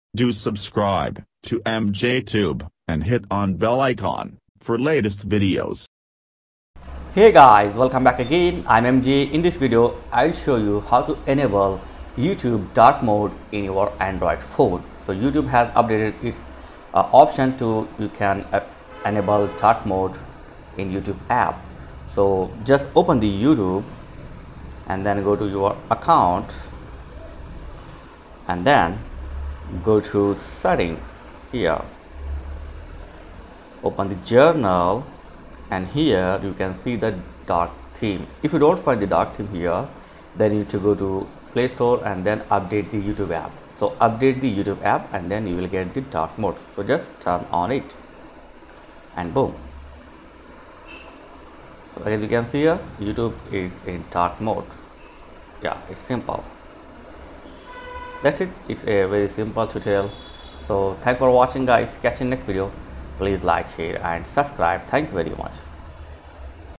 How to Enable Youtube Dark Mode in Android Phone & Tablet
Apply dark theme on youtube app